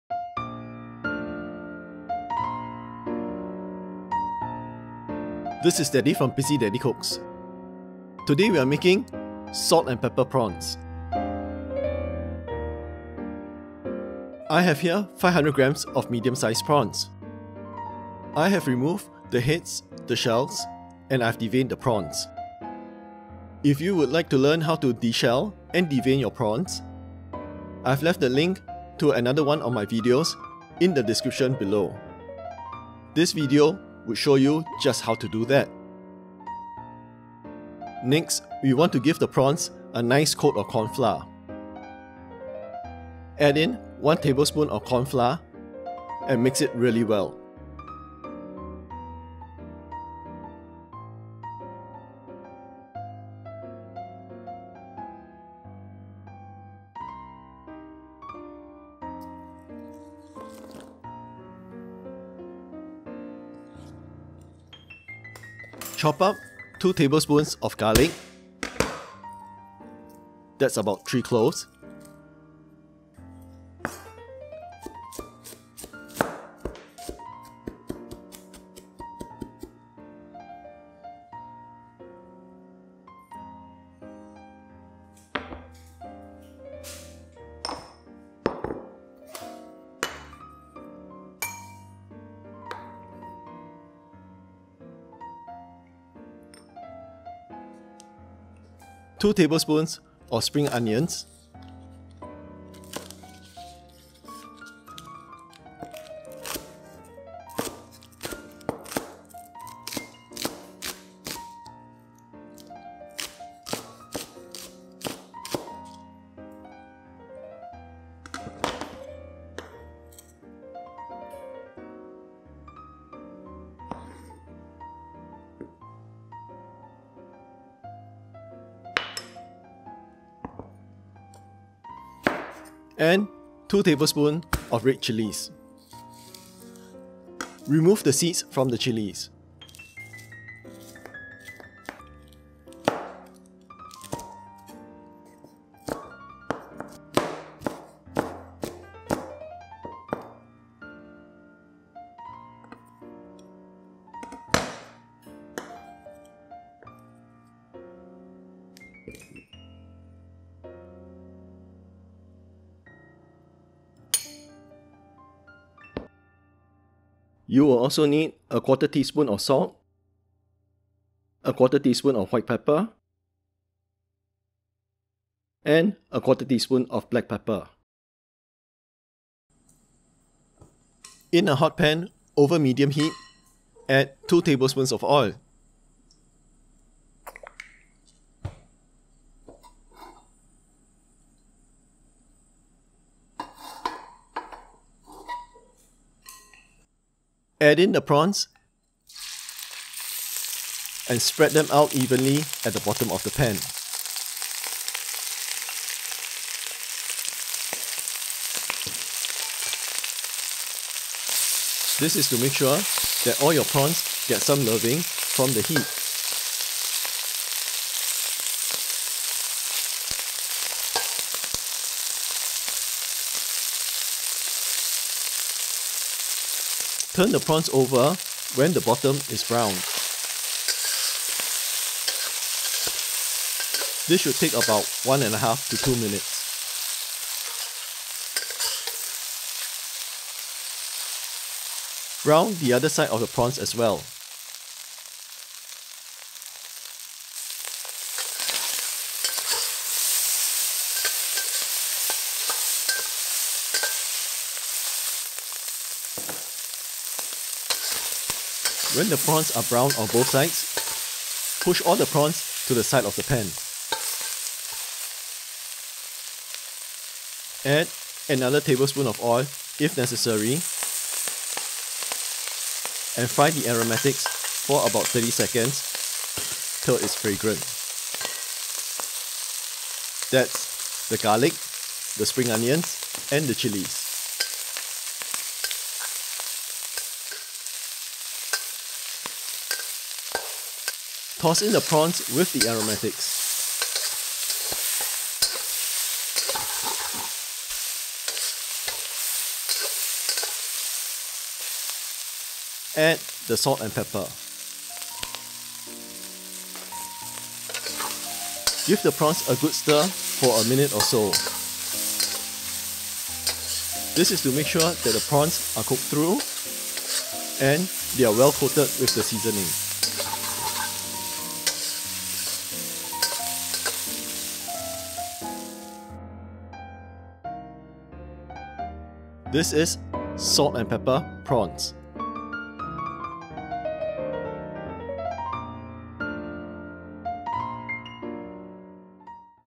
Salt And Pepper Prawns Recipe
What can be easier than salt and pepper? Salt and Pepper Prawns is one of those quick dishes that you can whip up in minutes. The aromatics give the dish its flavour while the salt and pepper enhance the taste.

Ingredients:
500g prawns, medium
2 Tbsp cornflour
2 Tbsp oil
2 Tbsp garlic, chopped
2 Tbsp spring onions, chopped
2 Tbsp red chilli, chopped

Seasoning:
1/4 tsp salt
1/4 tsp black pepper
1/4 tsp white pepper